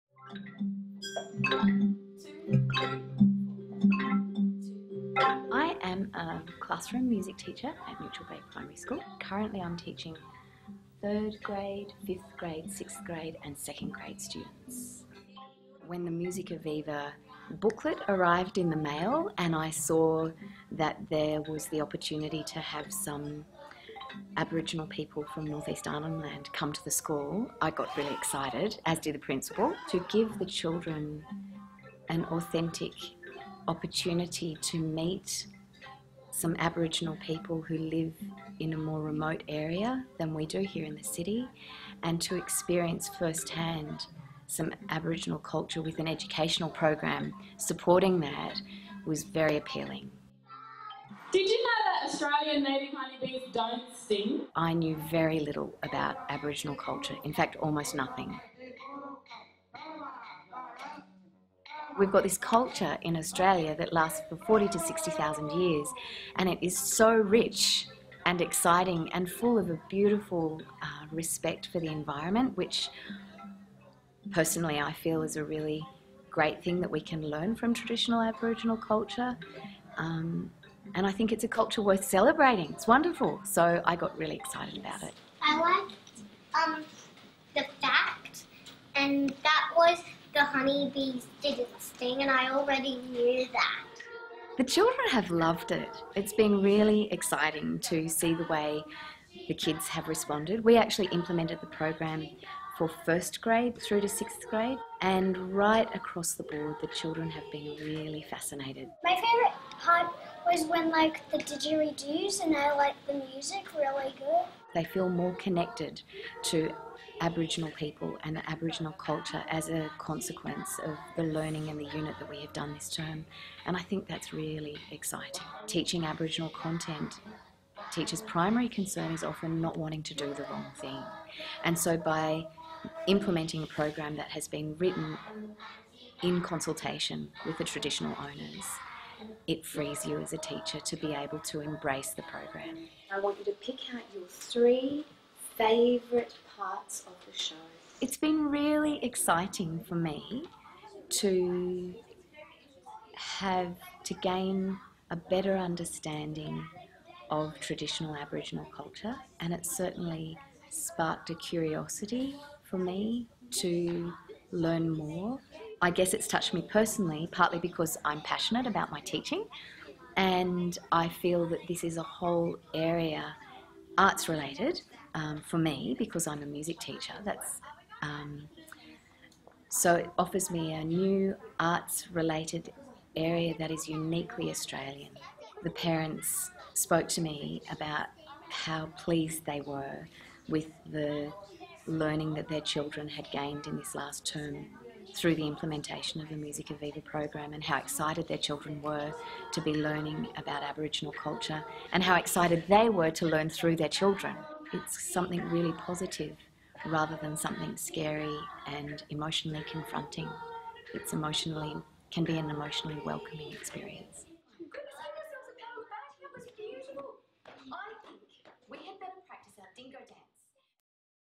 Music Education & Me - Mandy Langlois
Mandy Langlois, primary school teacher at Neutral Bay Public School, shares her experience of Musica Viva In Schools' education resource Live Performance Plus featuring Indigenous Australian ensemble Datiwuy Dreaming.